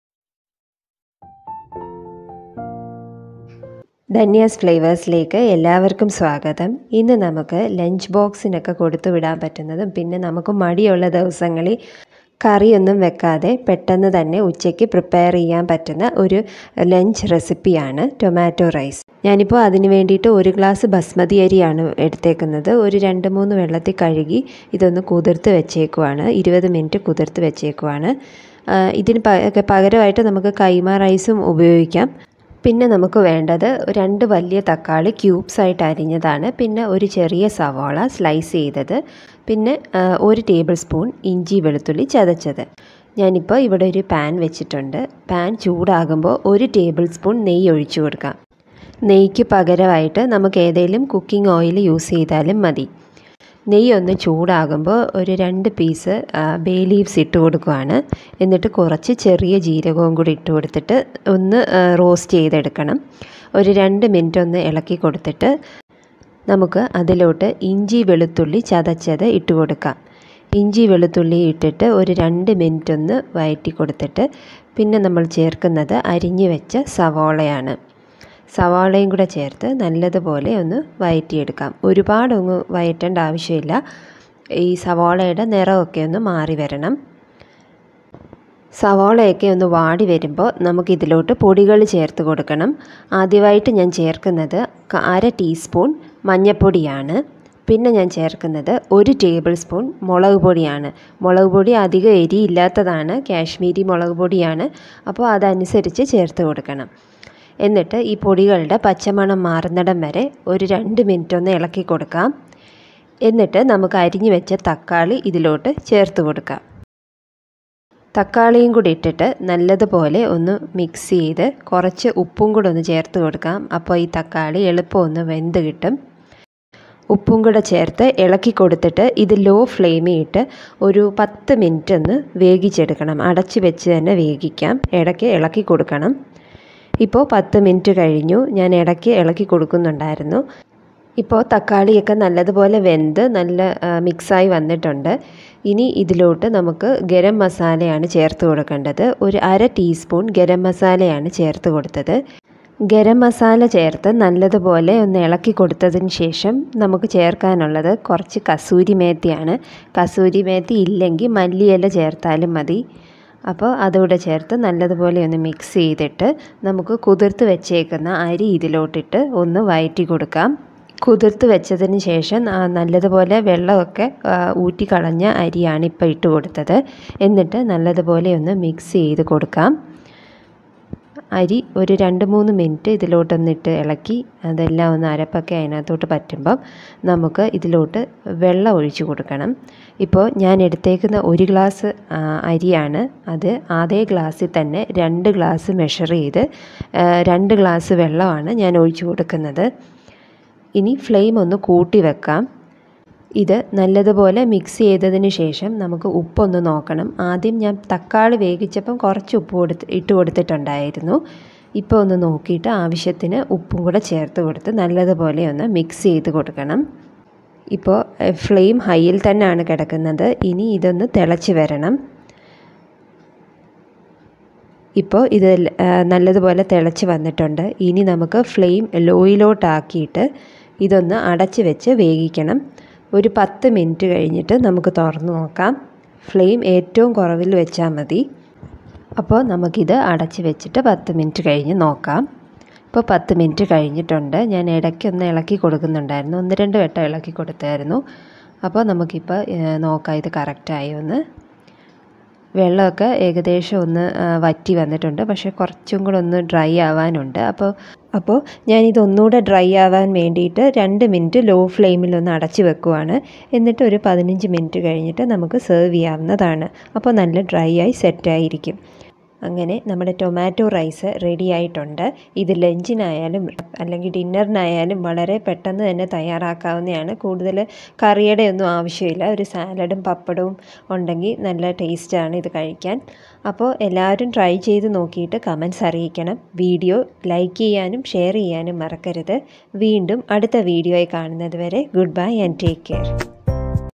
Bachelor's special /kids lunch box- Easy tomato rice Ep 31
Tomato rice

Ingredients

Basmati rice/jeera rice 1cup
Ginger/Garlic  1tbsp
Corriander leaves
Ghee/vegetable oil

Preparation

Heat oil or ghee in a pan. To this add bay leaves and cumin seeds.

Saute for a while and add crushed ginger garlic to it. Saute for abt a minute and add the slices onions.

When the onions become translucent add turmeric, Chilli powder and garam masala. Saute till the raw smell of the powders goes away.

To this add chopped tomatoes and add a pinch of salt so that the tomatoes get cooked faster. Cook in low flame for 10 minutes till the tomatoes are mashy. Add corriander leaves or kasuri methi.

To this add the soaked drained rice and mix well. Add water to double the cup of rice and bring to boiling in high flame.

When it starts boiling lower the flame and cook covered for 10-12 minutes.

Serve with salad,  pickle and pappad